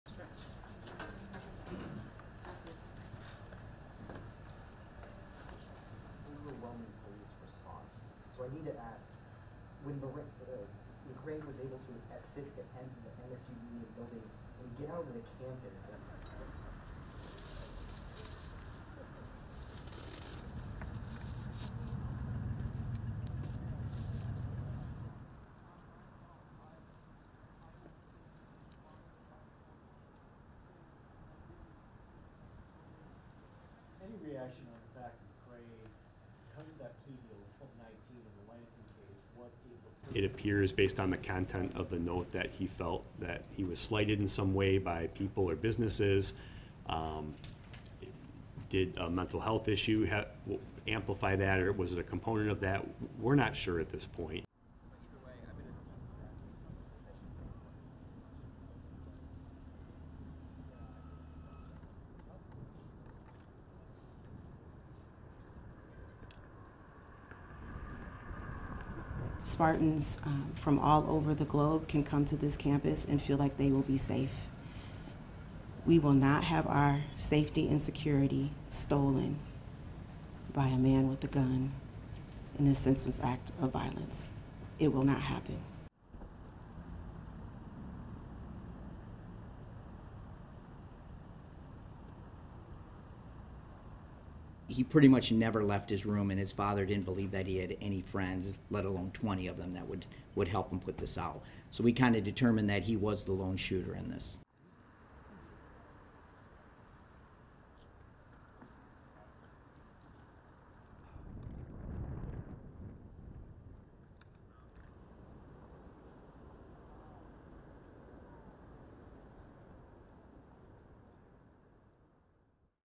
Police say Michigan State shooter felt 'slighted'
RESTRICTION SUMMARY:
ASSOCIATED PRESS
East Lansing, Michigan - 16 February 2023
1. Officials arrive at the site of a news conference

HEADLINE: Police say Michigan State shooter felt ‘slighted’

ASSOCIATED PRESS
2. Michigan State Deputy Police Chief Chris Rozman listens to a reporter's question

ANNOTATION: Police say the man who shot eight students at Michigan State University was found with a note containing a possible motive for the attack.

ASSOCIATED PRESS
ARCHIVE: Lansing, Michigan - 14 February 2023
3. Various of a Michigan State Police trooper affixing caution tape to a post near the site where Anthony McRae killed himself

ANNOTATION: The two-page note was found with Anthony McRae when he killed himself Monday night after being confronted by police.

ASSOCIATED PRESS
FILE: East Lansing, Michigan - 14 February 2023
4. A person leaves flowers at a makeshift memorial at the Spartan statue on Michigan State University’s campus

ANNOTATION: Investigators said Thursday they still were trying to pin down a motive, three days after the violence at the 50,000-student campus in East Lansing.

ASSOCIATED PRESS
5. Rozman listens to a question

ANNOTATION: But the note was a key point.

ASSOCIATED PRESS
6. SOUNDBITE (English) Interim Deputy Chief Chris Rozman, Michigan State University police:
++PARTIALLY COVERED++
"It appears, based on the content of the note, that he felt that he was slighted in some way by people or businesses. Did a mental health issue amplify that, or was it a component of that? We're not sure at this point."

ASSOCIATED PRESS
7. Flowers piled up at the makeshift memorial

ANNOTATION: The shootings took place at Berkey Hall, an academic building, and the MSU Union.

ASSOCIATED PRESS
8. Various of mourners on campus

ANNOTATION: Students were ordered to shelter in place for four hours while police hunted for the gunman.

ASSOCIATED PRESS
9. SOUNDBITE (English) Rema Vassar, Michigan State University Board of Trustees chair:
++PARTIALLY COVERED++
“Spartans from all over the globe can come to this campus and feel like they will be safe. We will not have our safety and security stolen by a man with a gun in a senseless act of violence. It will not happen.”

ASSOCIATED PRESS
10. The site where McRae killed himself

ANNOTATION: According to police, McRae walked nearly 4 miles toward his Lansing home after the shootings and said nothing before killing himself.

ASSOCIATED PRESS
11. SOUNDBITE (English) Lt. Rene Gonzalez, Michigan State Police:
++PARTIALLY COVERED++
“He pretty much never left his room. And his father didn’t believe that he had any friends, let alone 20 of them that would help him put this out. So, we kind of determined that he was the lone shooter in this."

ASSOCIATED PRESS
12. Various of mourners at the statue

ANNOTATION: One of the five injured students was upgraded to stable condition.

ANNOTATION: Classes remain suspended through the weekend. Berkey Hall will stay closed through the spring term.
STORYLINE:

The shootings took place at Berkey Hall, an academic building, and the MSU Union.